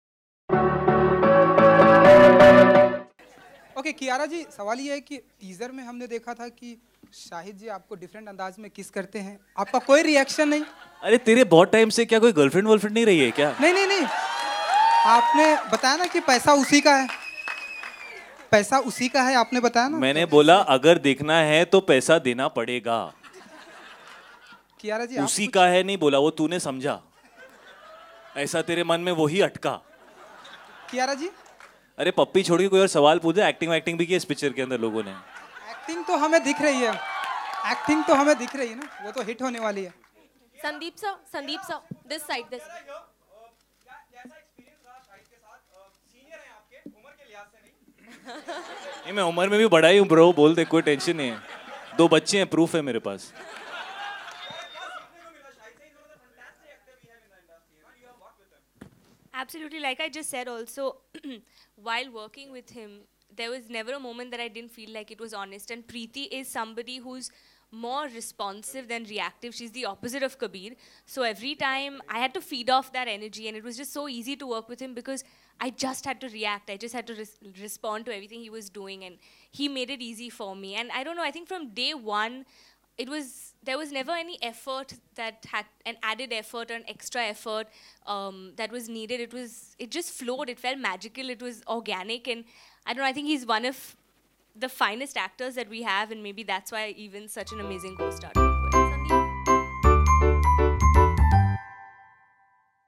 Shahid Kapoor INSULTS Reporter Asking About His Kiss With Kiara Advani
Shahid Kapoor made fun of a reporter at the 'Kabir Singh' trailer launch who asked about his kissing scene with Kiara Advani in the movie.